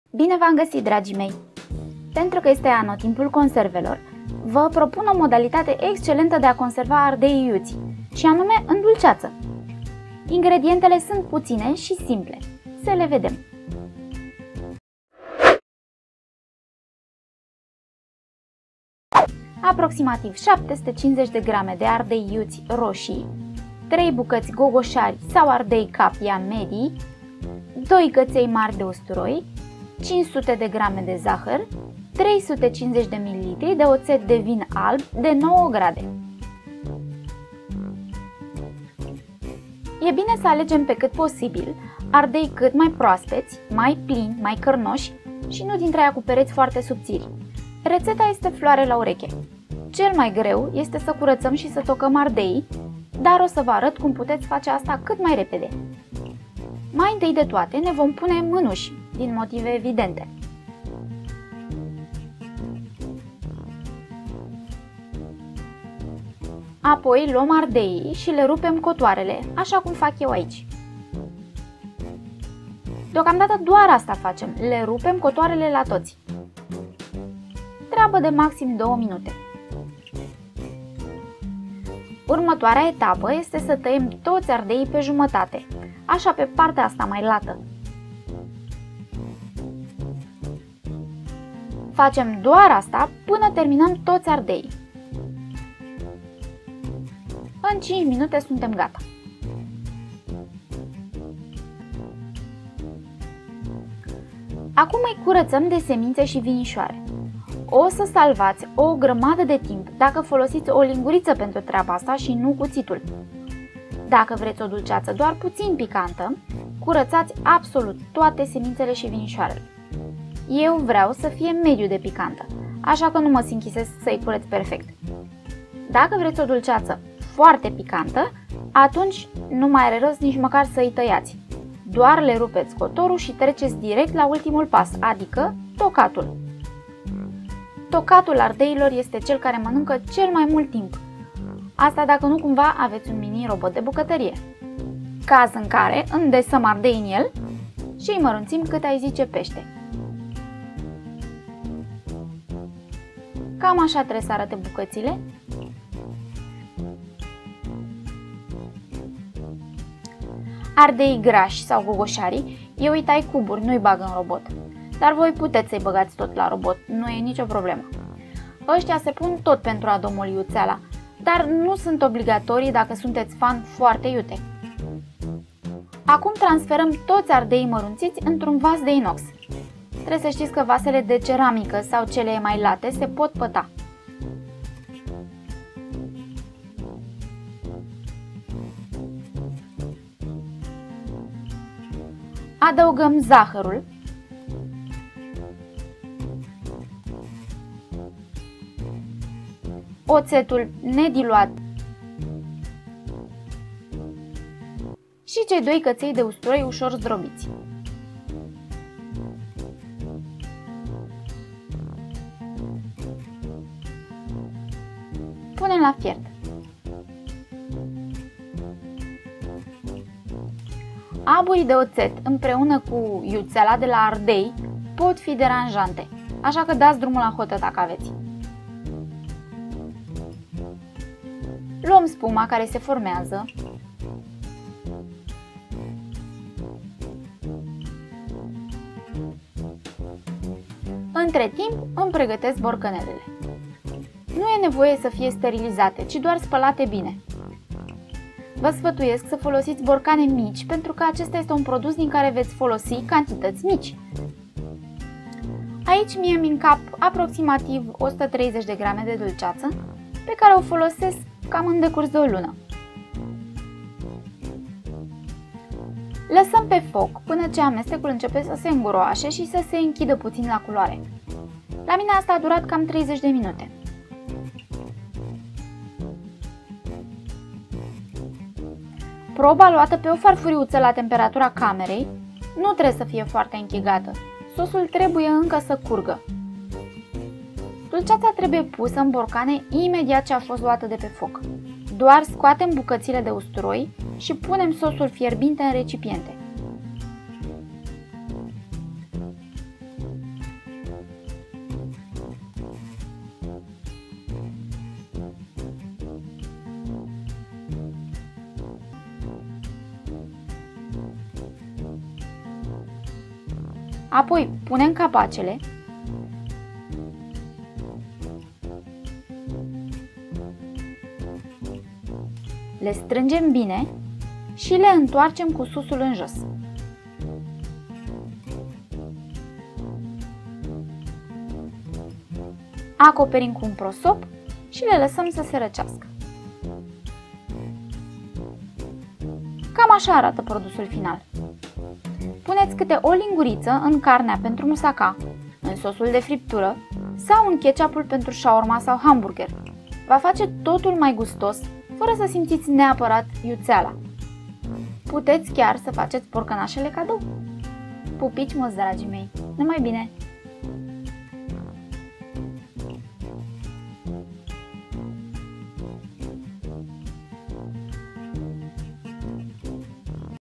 Ardei iute pentru iarna conservat altfel - Dulceata de ardei iuti
Toti punem conserve pentru iarna. Va propun sa iesiti putin din tipar si pe langa ardei iute la borcan, sa faceti si niste dulceata de ardei iute cu capia sau cu gogosari. Sunt sigura ca ii veti gasi multe intrebuintari. Ardei iute in otet